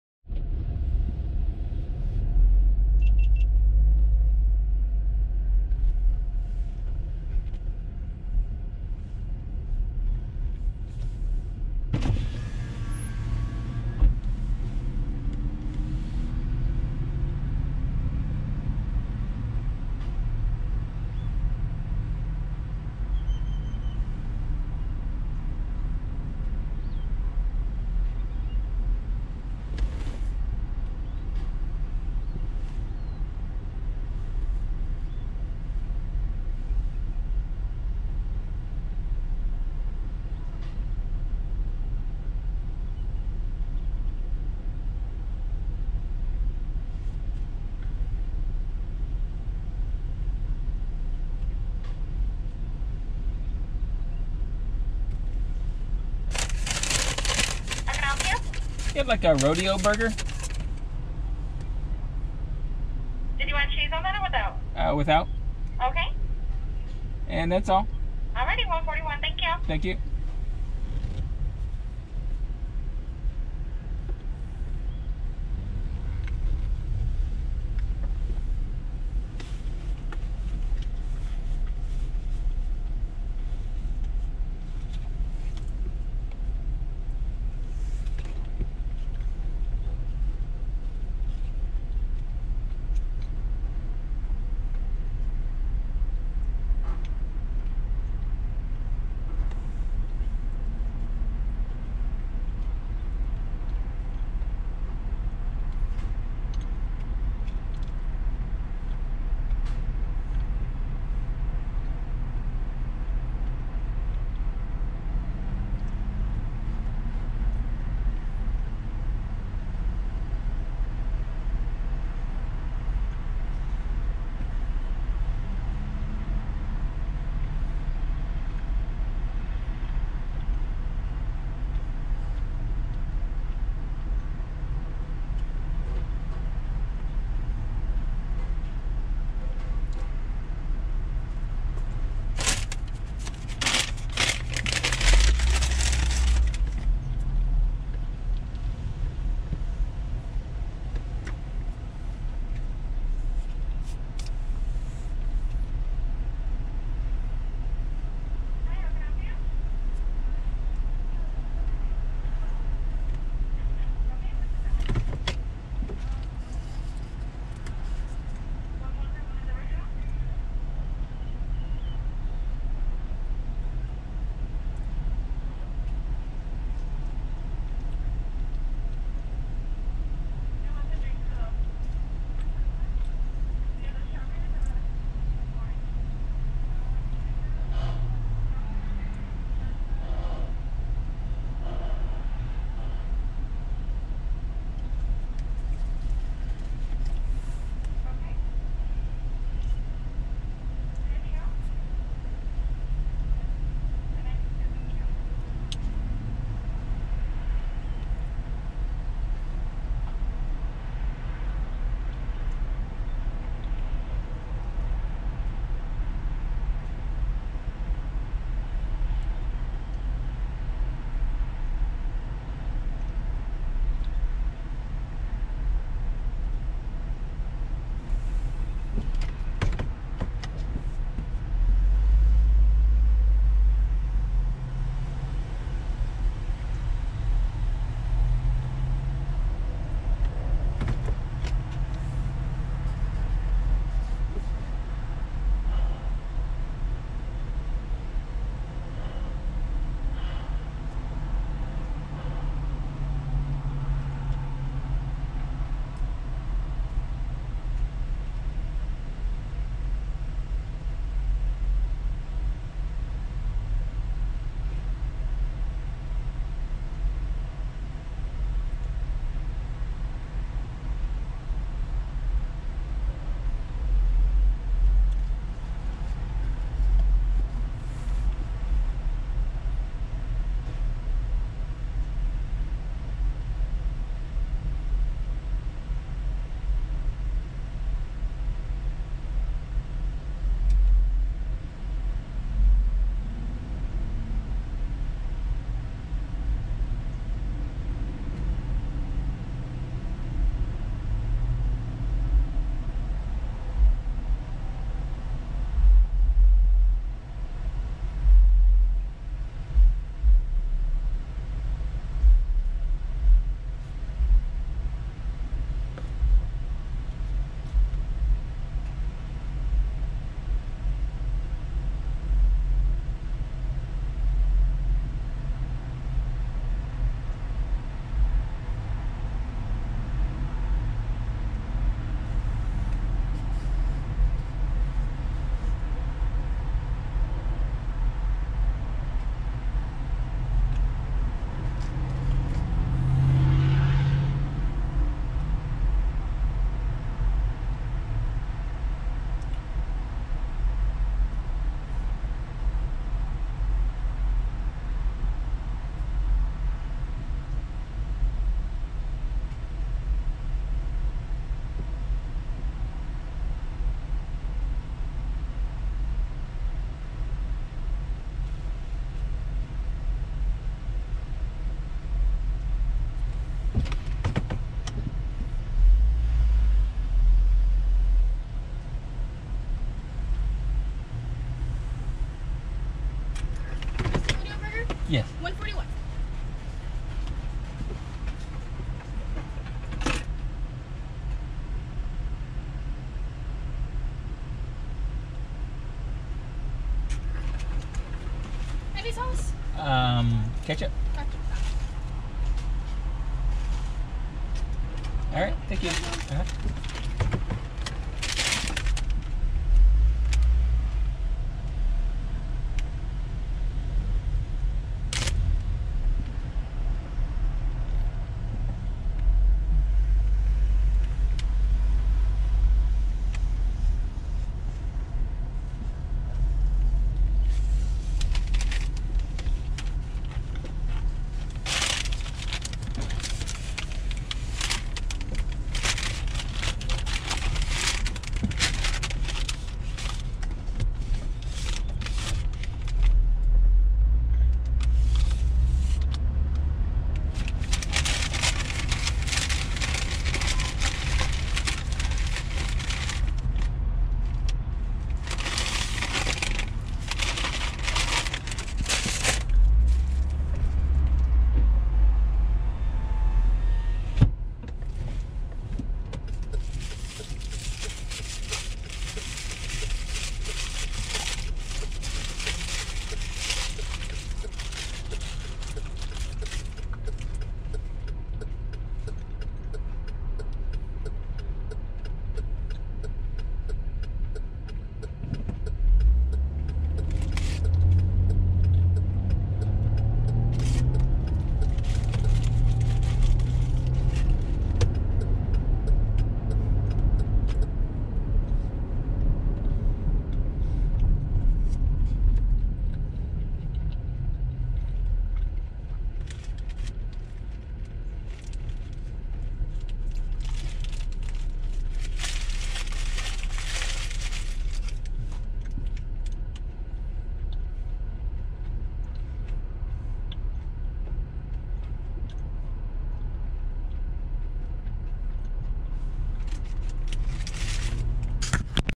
Burger King Drive-Thru, Rodeo Burger ala Bavarian Kreme, Maricopa, Arizona, 13 January 2022 GX019009
$1.41 Rodeo Burger without Cheese,
Burger King, 20699 N John Wayne Pkwy, Maricopa, AZ, 85139,